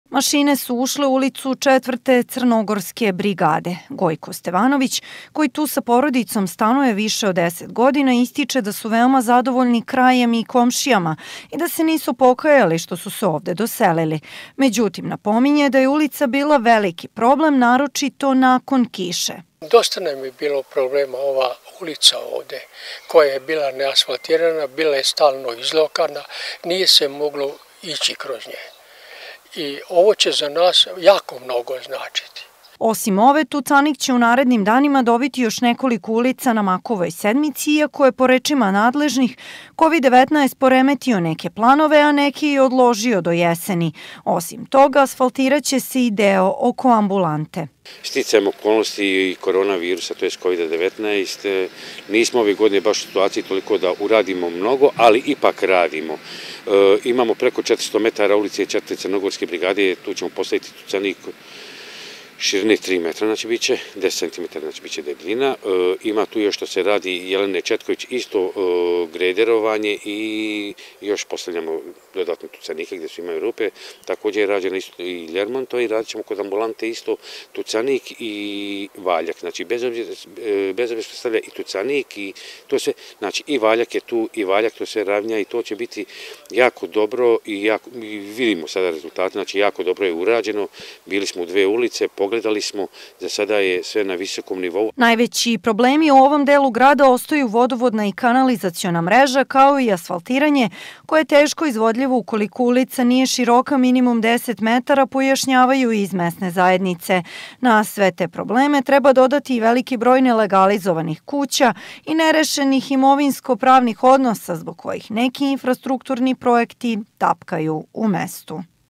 Tucanik u nekoliko ulica na Makovoj sedmici
Počelo je posipanje tucanika i grederovanje u nekoliko ulica na Makovoj sedmici. Iz Mesne zajednice poručuju da radovi malo kasne zbog situacije sa kovidom-19, ali da su zadovoljni što kako se odvijaju.

Mašine su ušle u Ulicu IV crnogorske brigade. Gojko Stevanović, koji tu sa porodicom stanuje više od deset godina, ističe da su veoma zadovoljni krajem i komšijama i da se nisu pokajali što su se ovde doselili. Međutim, napominje da je ulica bila veliki problem, naročito nakon kiše.

Osim ove, tucanik će u narednim danima dobiti još nekoliko ulica na Makovoj sedmici, iako je, po rečima nadležnih, kovid-19 poremetio neke planove, a neke i odložio do jeseni. Osim toga, asfaltiraće se i deo oko ambulante.

Najveći problemi u ovom delu grada ostaju vodovodna i kanalizaciona mreža, kao i asfaltiranje, koje je teško izvodljivo ukoliko ulica nije široka minimum deset metara, pojašnjavaju iz mesne zajednice.Na sve te probleme treba dodati i veliki broj nelegalizovanih kuća i nerešenih imovinsko-pravnih odnosa, zbog kojih neki infrastrukturni projekti tapkaju u mestu.